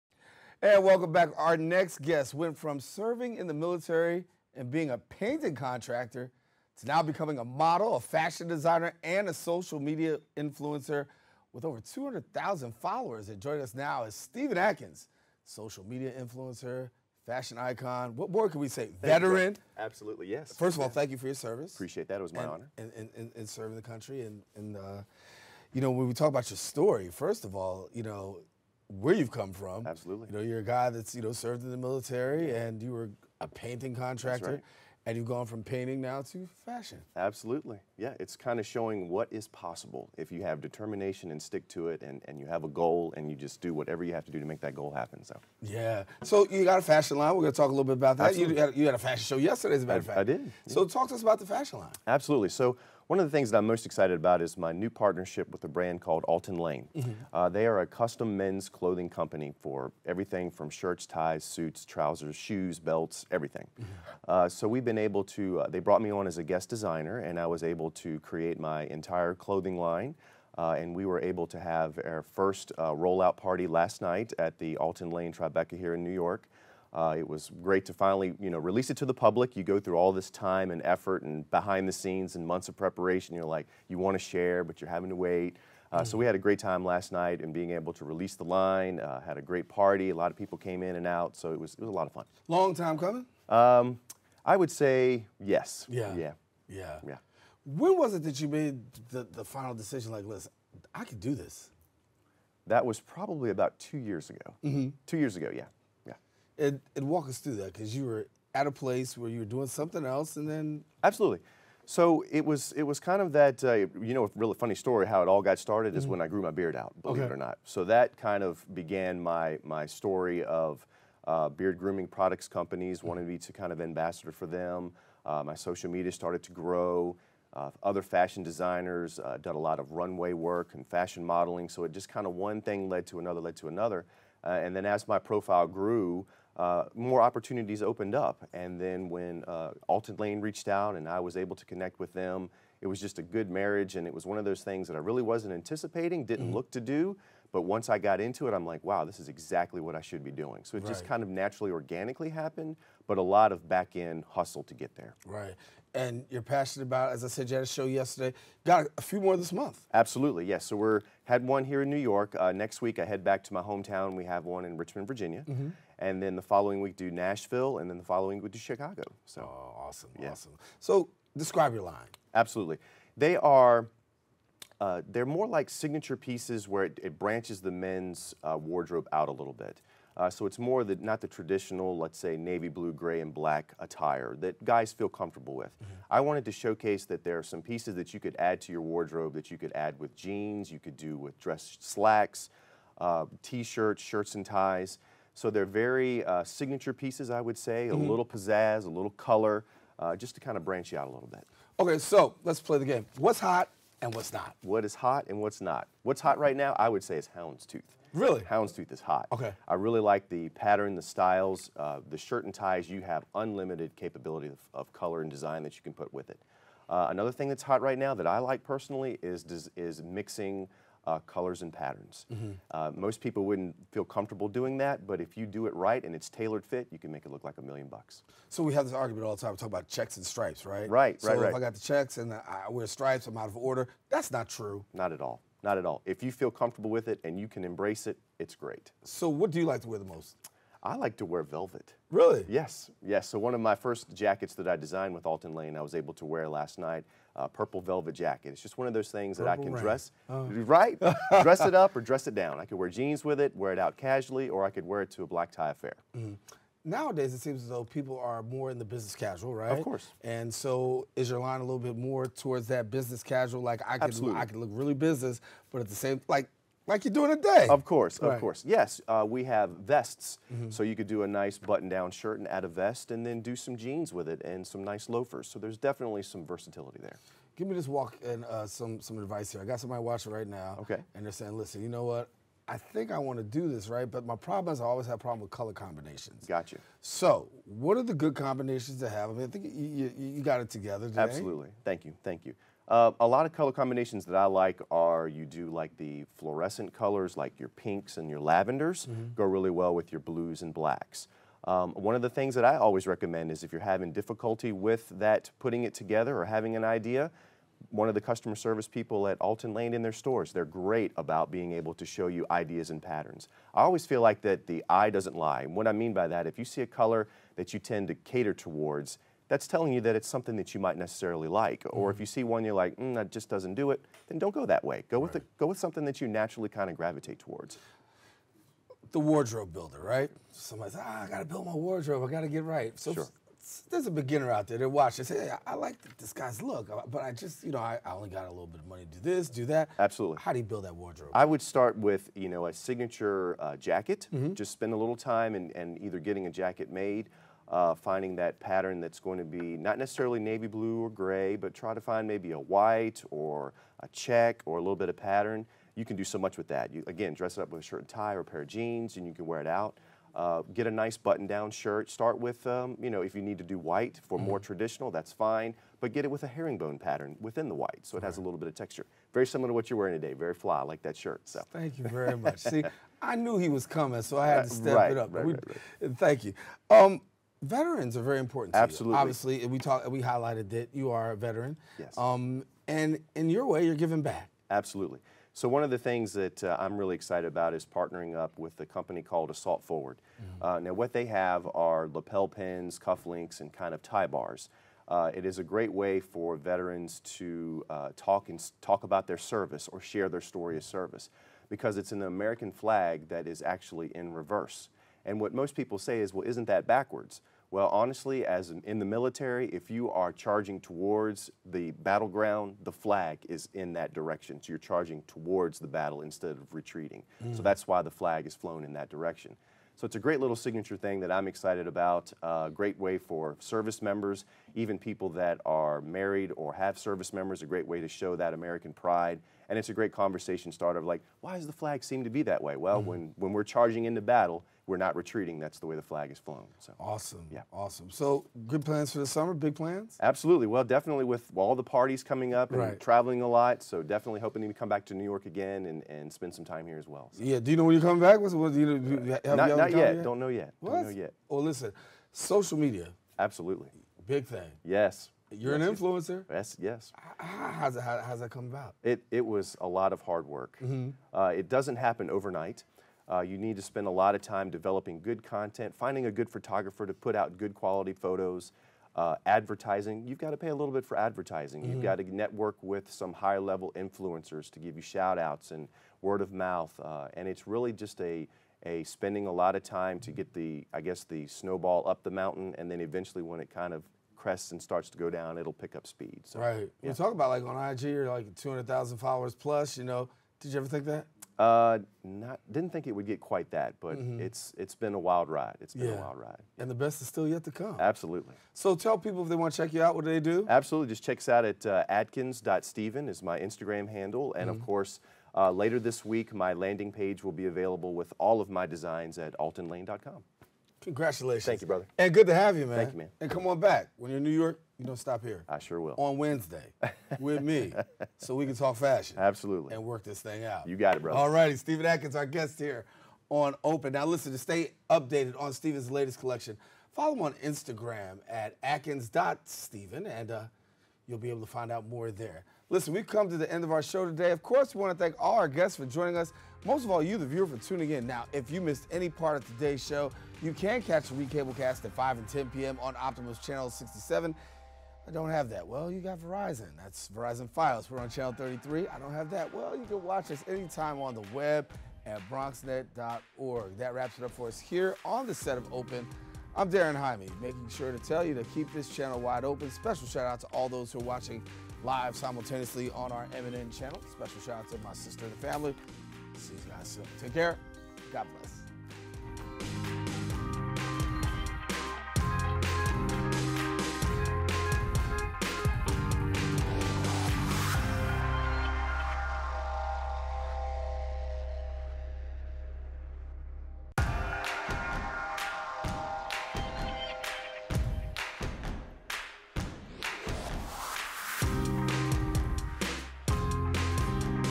OPEN: Fashion Influencer Stephen Adkins
Host Daren Jaime talks with Social Media Influencer, Entrepreneur, Veteran and Model, Stephen Adkins speaks about building his own brand and his recent guest-designer event with Alton Lane.